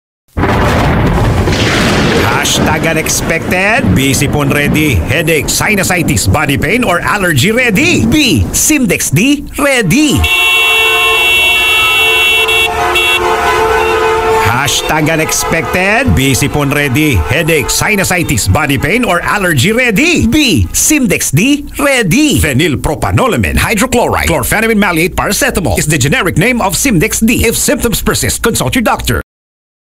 Symdex-D New Radio Ad (2019 - 2021) “Thunderstorm and Busina" (Voiced by Dennis Antenor Jr.)
Phenylpropanolamine hydrochloride, chlorphenamine maleate, paracetamol is the generic name of Symdex-D, If symptoms persist consult your doctor.
Symdex-D for UnexpectedSiponHeadacheSinusitisBodyPainOrAllergy BeSymdexDReady, If symptoms persist consult your doctor.